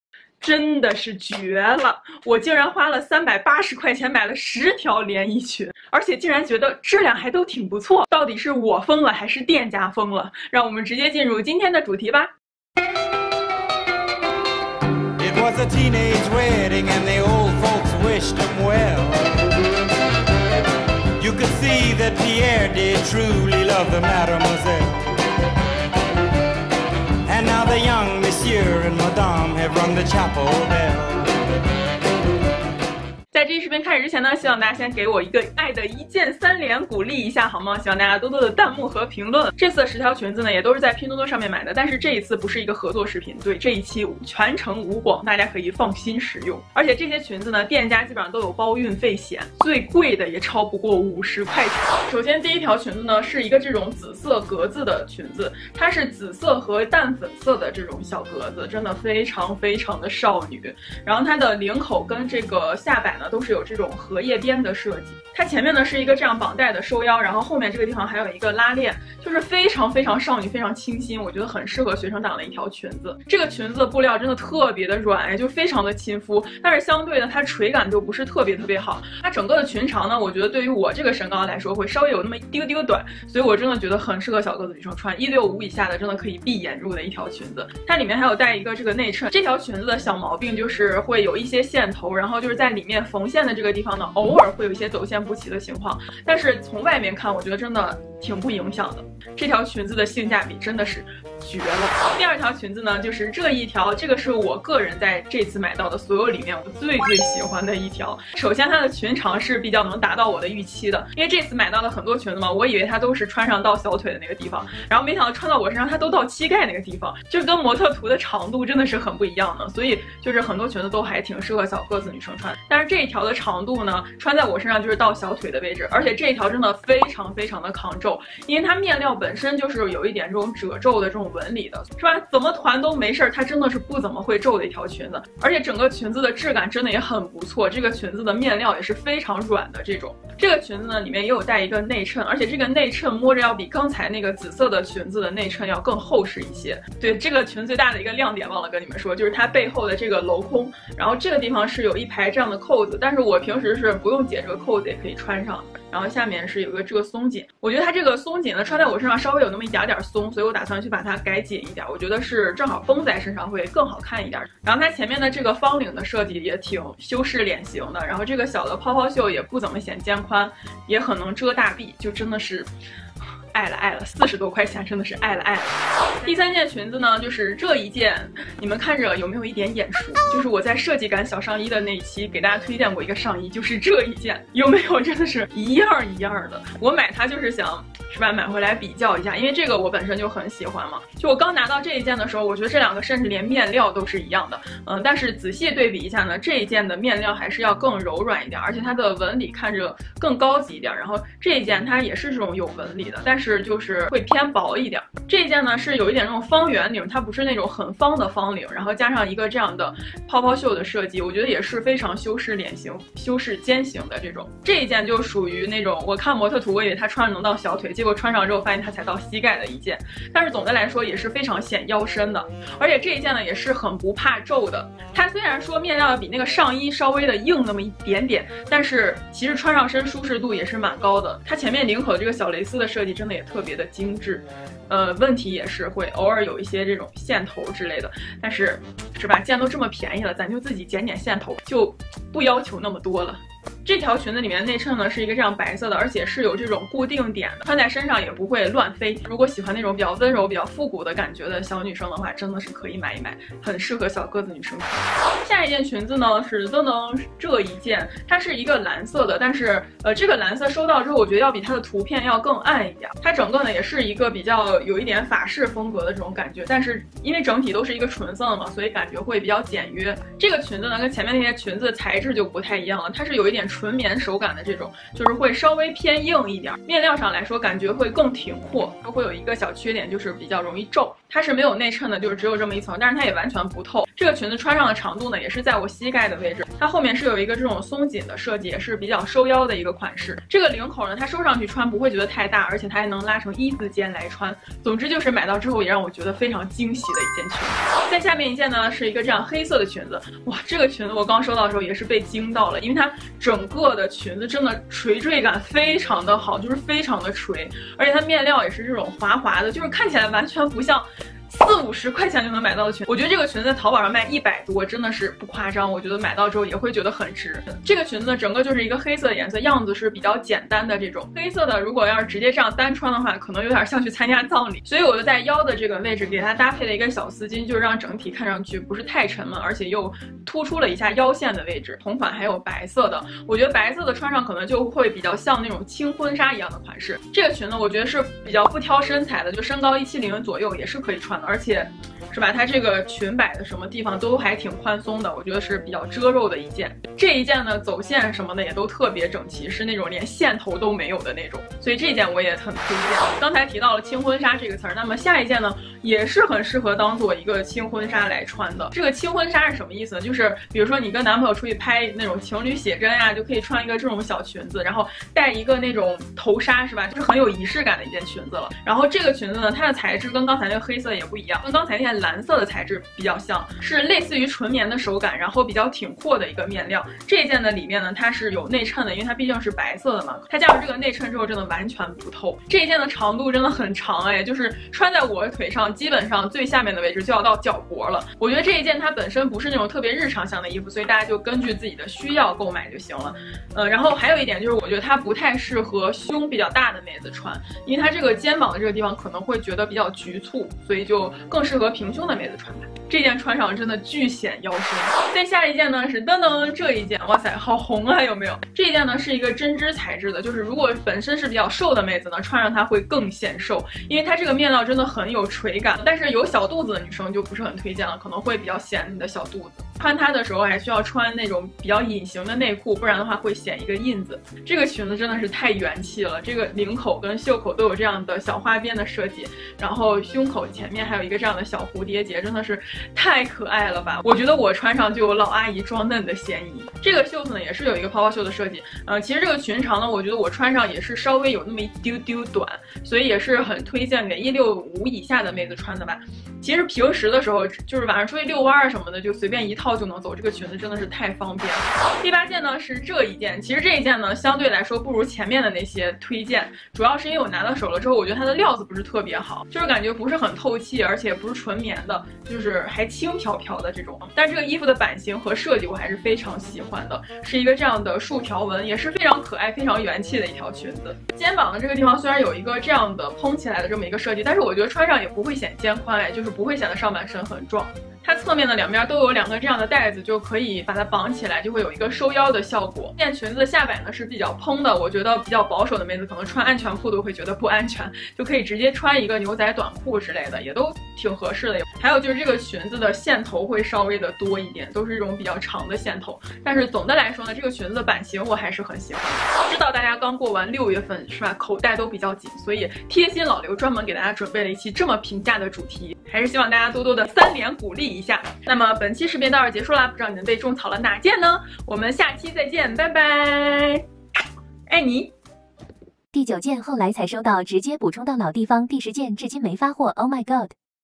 绝了！均价38元，9条连衣裙分享，还包运费险那种！
身高168 体重56 肩宽40 腰围70仅供参考